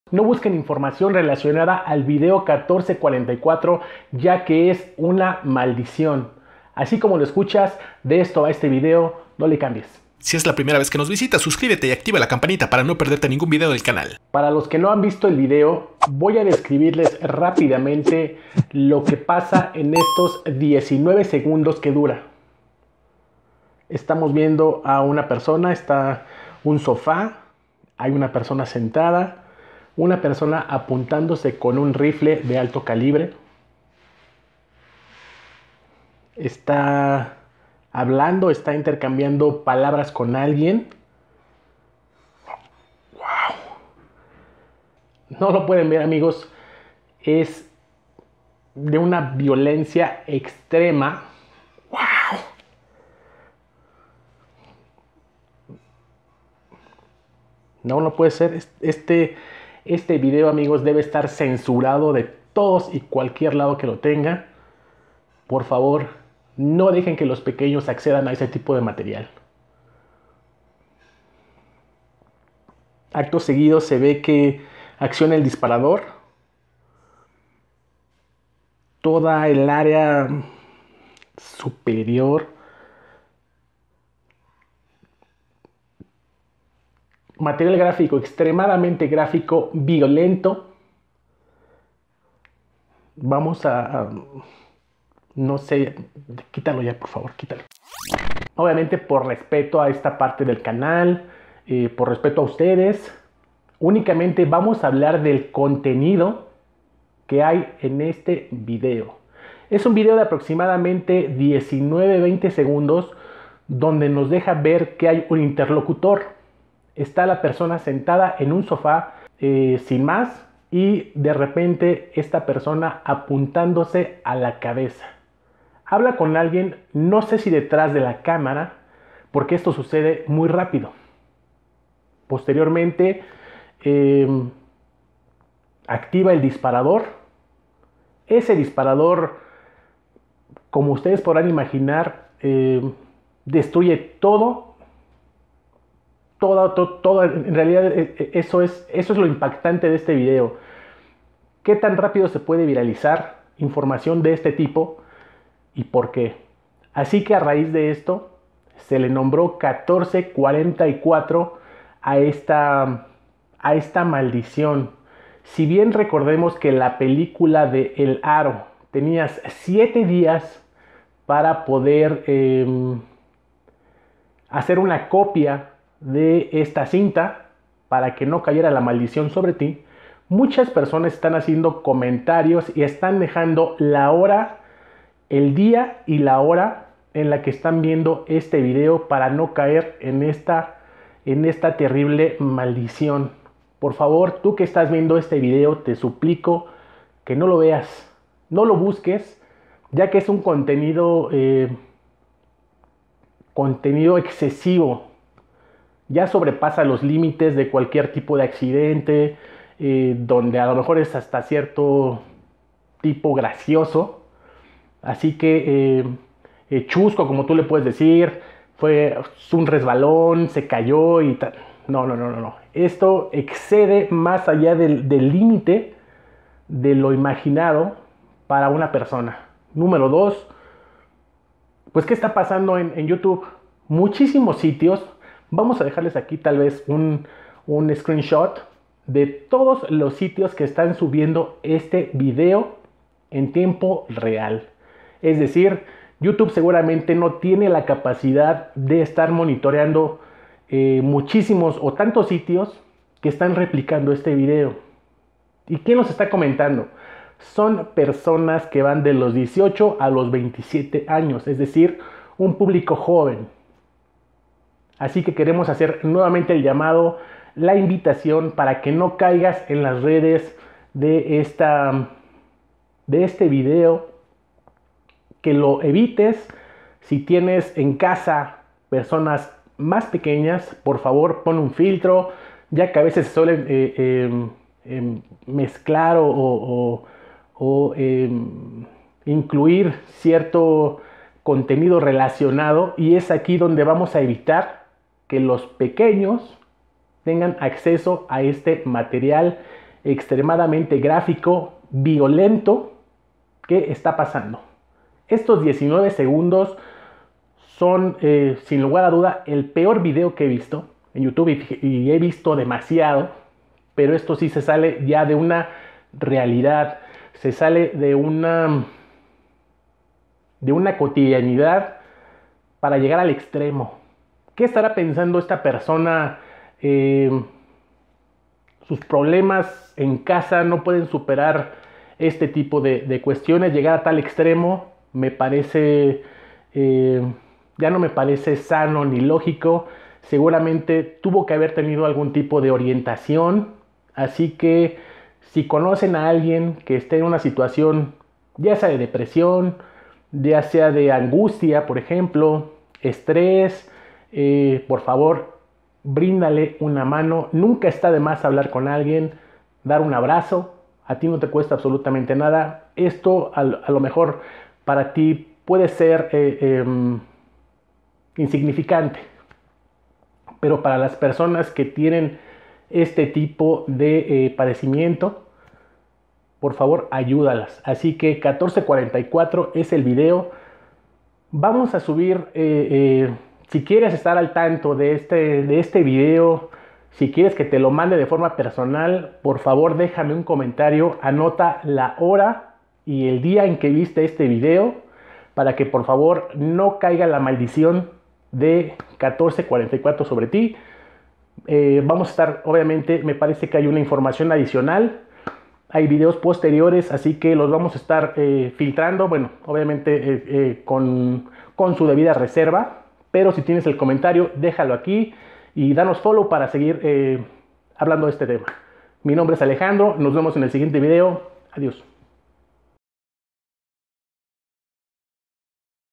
1444 Like si no quieres ☠ - aterrador - deep web - reacción
1444 es el aterrador video que ha sido comentado por todos los youtubers como  el venezolano DrossRotzank, mejor conocido como Dross, quién fijó su postura respecto al contenido, varios portales de noticias como Heraldo de México, Sopitas, El Mundo.es., Mediotiempo, Multimedios TV, Noticieros Televisa.

Es un video que dura 20 segundos pero en él se puede observar a un ruso llamado Korablev Gleb Vyacheslavovich que fue identificado horas posteriores al video, se presume que fue transmitido en tiempo real en la deep web donde miles de personas lograron verlo. Se cuenta que Korablev pertenecía al Proyecto Juvenl 911.

Muchos sitios en youtube han corrido el riesgo de poder ponerlo en línea pero son bajados al instante, así mismo son temas tabú en la plataforma.

Se dice que si lo ves acarrea una tragedia, una maldición así como la película de El Aro, dónde tienes 7 días para poder realizar una copia o de lo contrario el fantasma traerá consigo una mala noticia.

Este video se ha vuelto viral rápidamente por su contenido, 1444 es el código clave, puedes ubicarlo en cualquier red social, tanto como facebook, twitter, te recomendamos no verlo sin supervisión.

Recuerda que si tecleas 1444 no entrar en este juego similar a la ballena blanca.

Recuerda poner en la caja de comentarios el día y la hora para que no caigas en esta maldición.